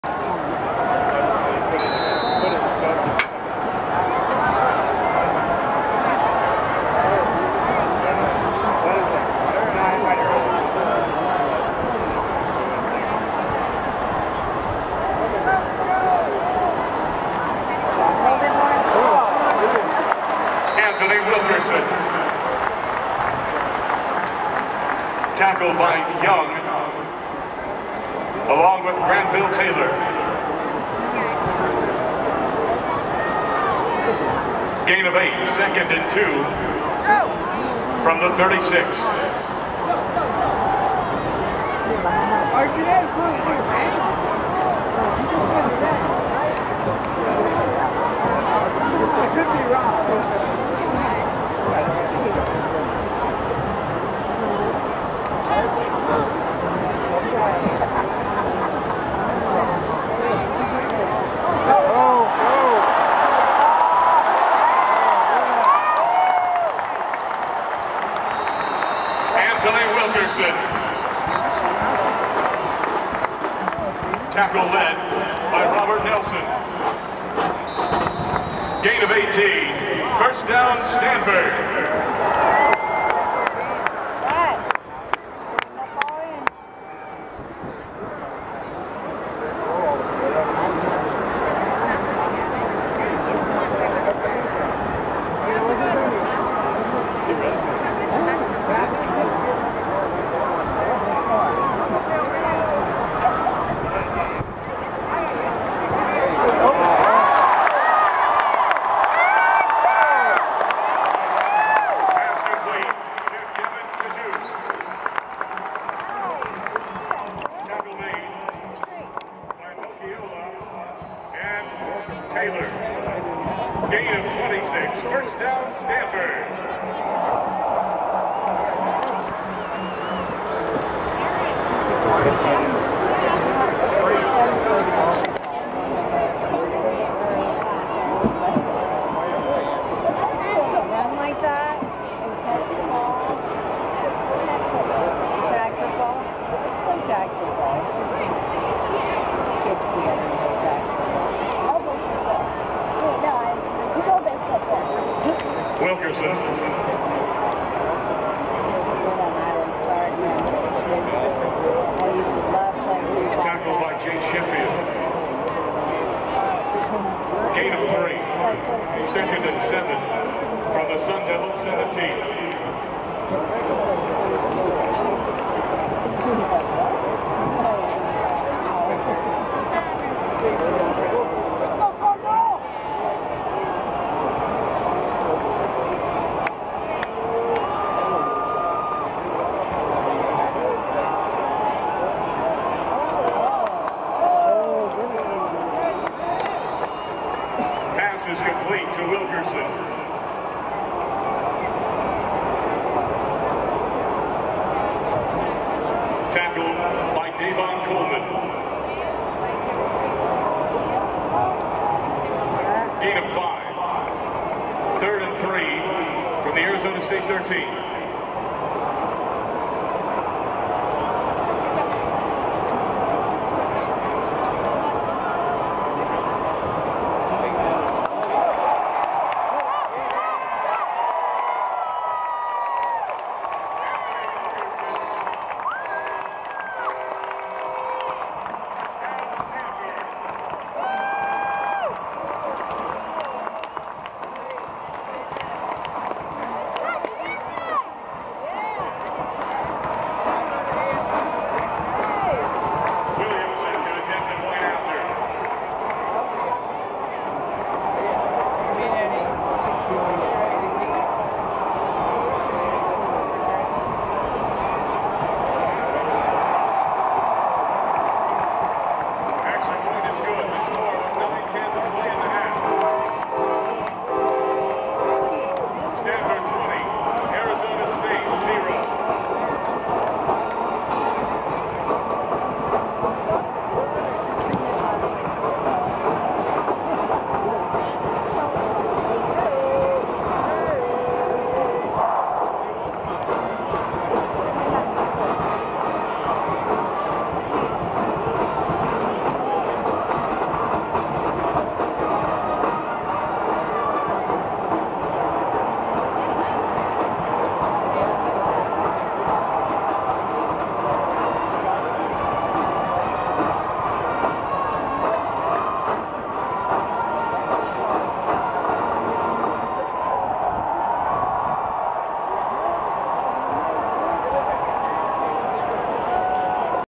Anthony Wilkerson 12-yard touchdown run Stanford vs. Arizona State Football 2013 Stanford Stadium
2nd quarter, 3rd and 2 at ASU 12, Anthony Wilkerson rush for 12 yards for a touchdown. Jordan Williamson extra point good. Stanford Cardinal vs. Arizona State Sun Devils at Stanford Stadium in Stanford, California on Saturday, September 21, 2013. Stanford won 42-28.